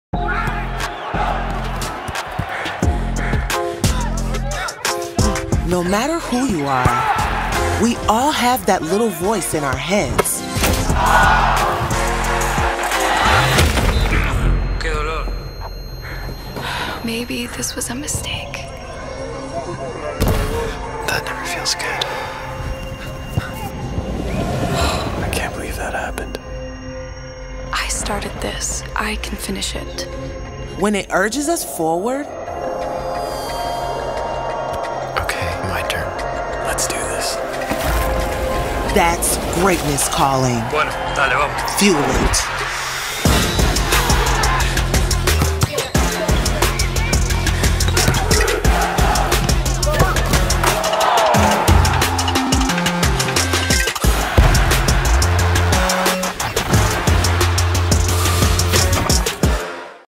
Gatorade - Voces interiores
Con material creativo que cuenta con la participación de Lionel Messi, la campaña ofrece una serie de programas globales para 2022 que incluyen el respaldo de jugadores aficionados con el regreso del torneo 5v5 y una nueva serie web en alianza con UEFA.